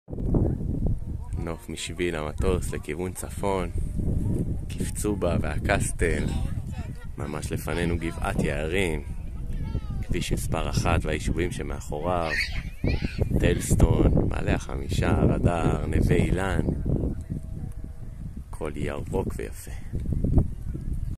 שביל המטוס - סובב איתנים - מבט לכיוון צפון
נוף מרשים מזרחה וצפונה, לאורך שביל המטוס (סובב איתנים) בהרי יהודה
צילום: ערן גל-אור
אפריל 2023

המסלול מפורט באפליקציית ׳מסלולים׳ ובאתר מסלולים, להתאהב בארץ מחדש.